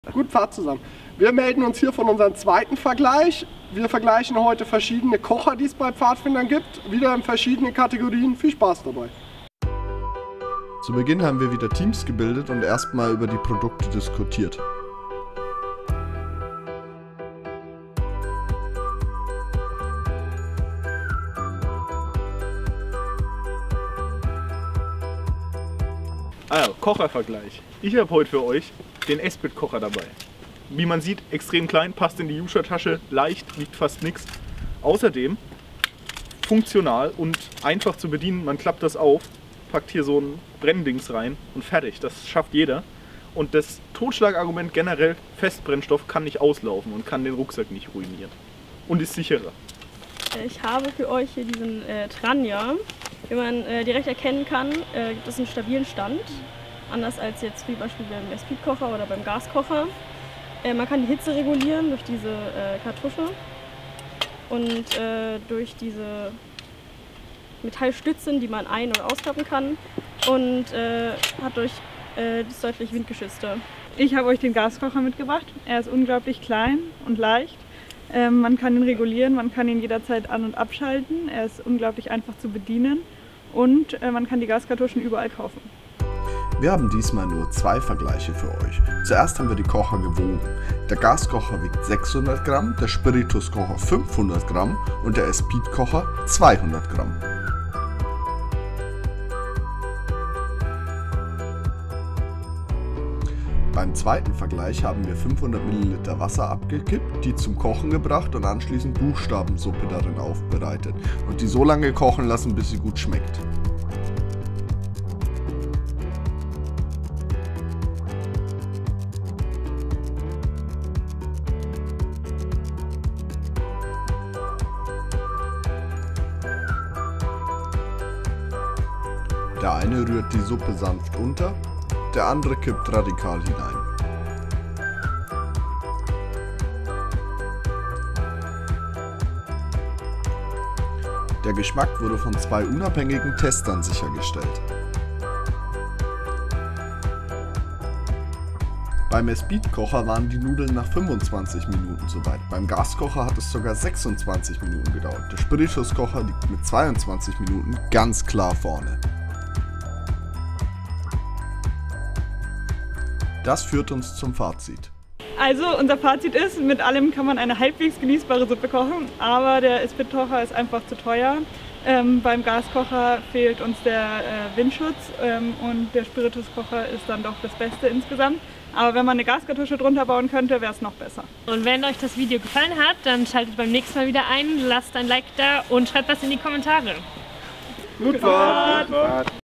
Esbit, Gas oder Spiritus - Der Vergleich | PBW
Hallo und Gut Pfad zusammen!  :)

Im heutigen Video zeigen wir euch den zweiten von drei Vergleichen, die wir für euch am Stammesführerwochenende aufgenommen haben.

Wir haben einen Esbitkocher, einen Gaskocher und einen Tranja (Spirituskocher) verglichen.

Wir hoffen euch gefällt das Video und wünschen euch viel Spaß beim Anschauen! ;)

Gut Pfad,
eure PbW-YouTube-Team! :)

Heute im Video:

- div Teilnehmer des Stammesführerwochenende

An der Kamera:

- Matze
- Sandra

Schnitt:

- Matze